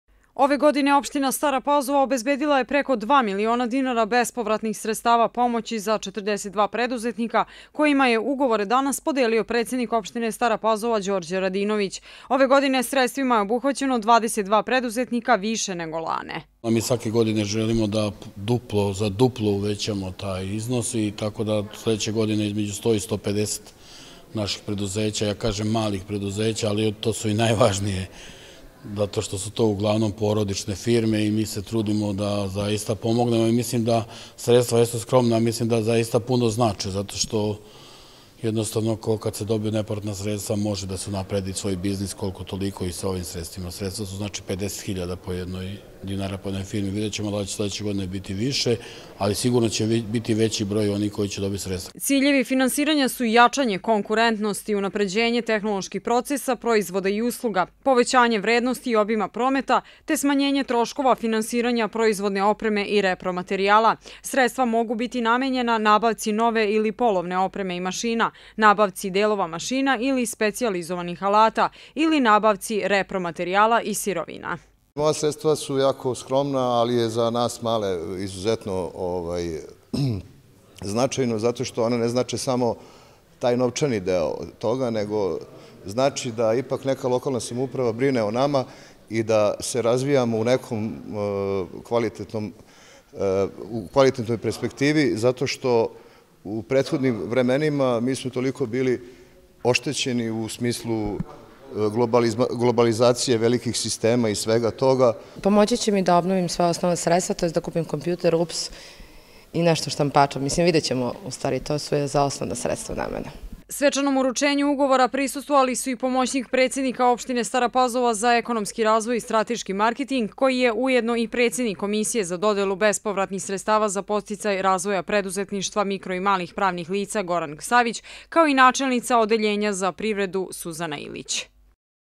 Pomoć opštine malim preduzećima
U Skupštinskoj sali opštine Stara Pazova svečano su uručeni ugovori svim preduzetnicima koji su ispunili kriterijume javnog konkursa opštine Stara Pazova. Ove godine bespovratna sredstva dobila su 42 preduzetnika.